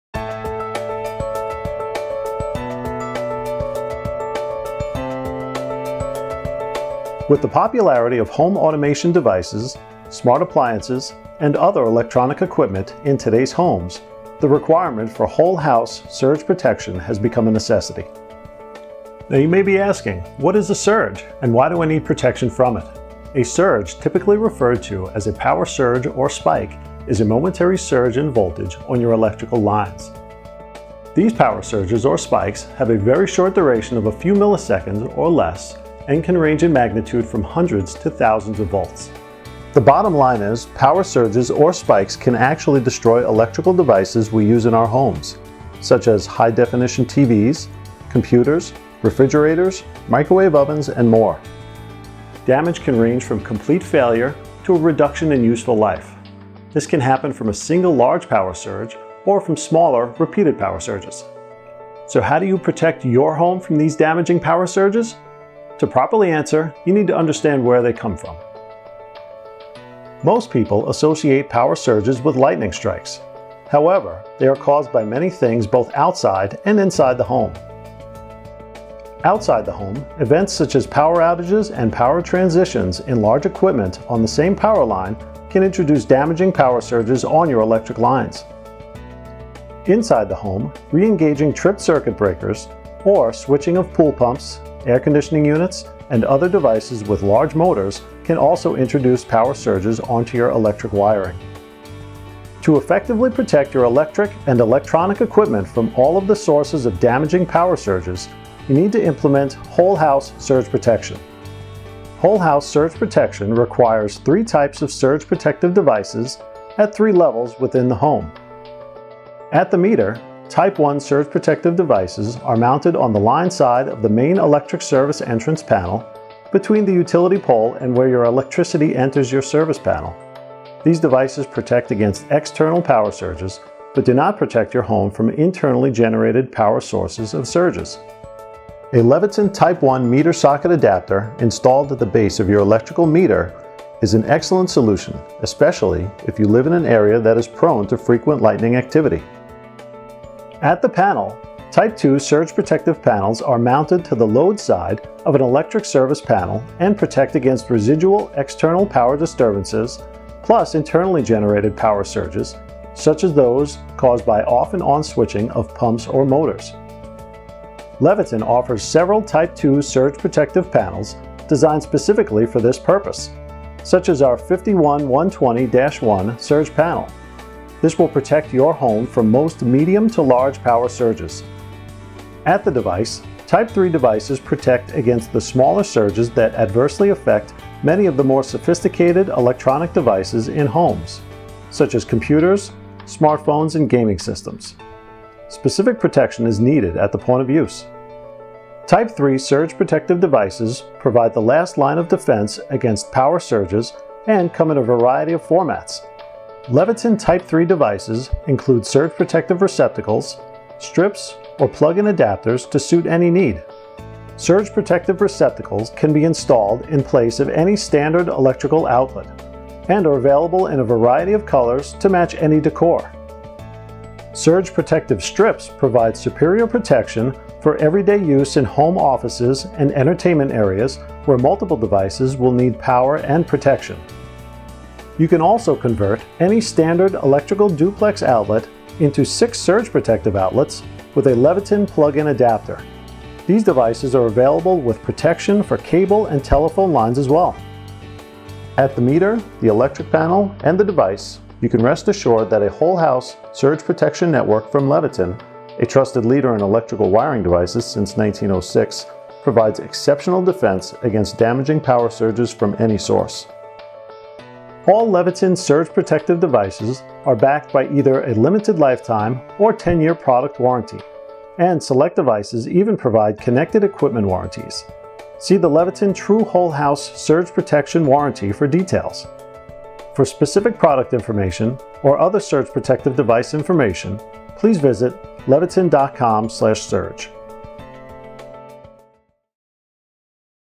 Whole House Surge Protection | Leviton
To some, whole house surge protection is achieved simply by installing a surge protective breaker or panel at the home's main breaker load center(s). However, true whole house surge protection requires surge protective devices at various levels within the home to provide the most reliable protection - a surge protection network if you will.

UL - a global independent safety company that tests for and certifies product safety - has categorized surge protective devices into various Types to indicate their specific performance characteristics and areas of usage. A true whole house surge protection network requires three Types of surge protective devices to effectively protect sensitive electrical and electronic equipment from the damaging effects of voltage surges.